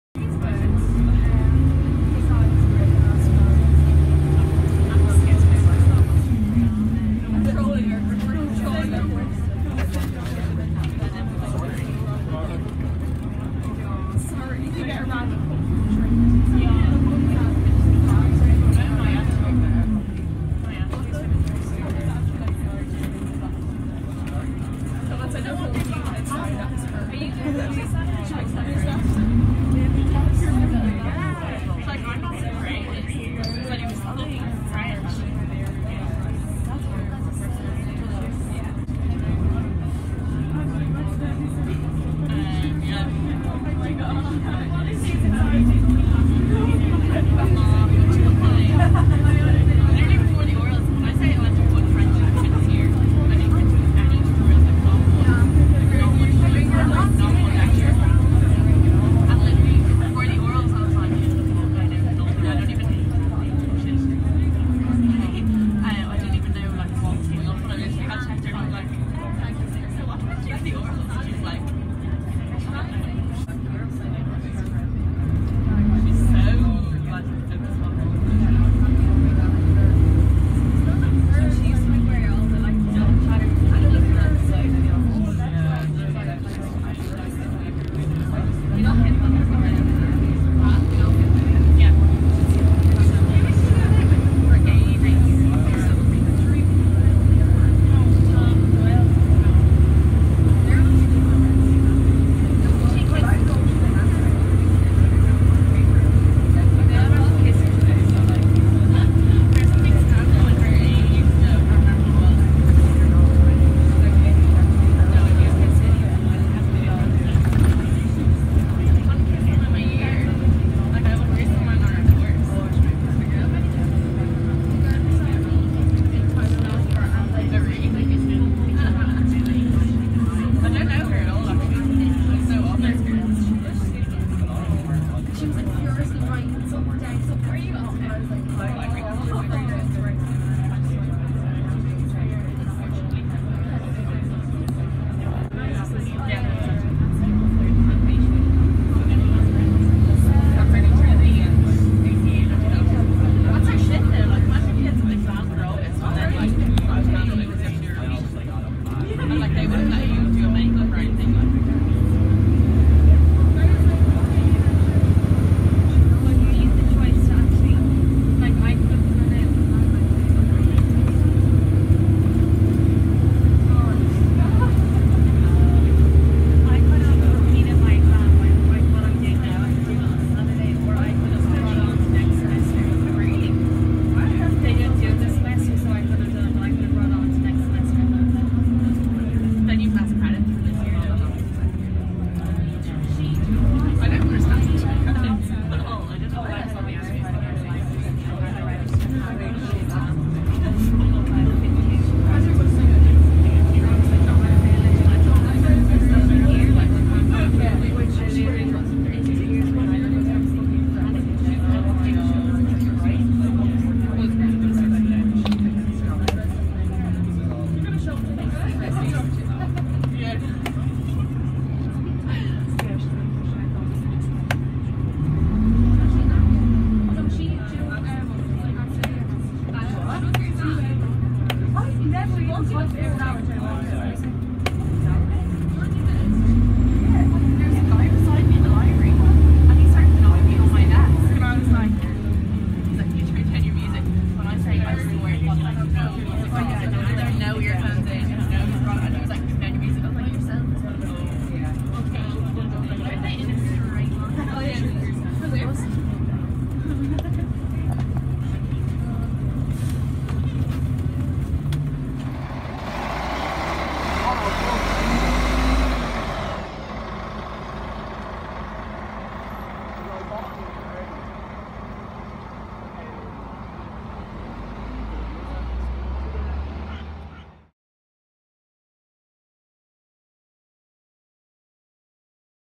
Reliance Coaches Volvo Olympian RH (99-D-473) | Former Dublin Bus RV473
I was lucky enough to have caught this on the UCD exam shuttles. It was lovely to have traveled on one of my childhood buses again, with thanks to the owner for letting me film and travel on this proper bit of kit 👌🏻

RV473 (99-D-473)
Volvo Olympian Alexander RH
Former Phibsboro & Ringsend Garage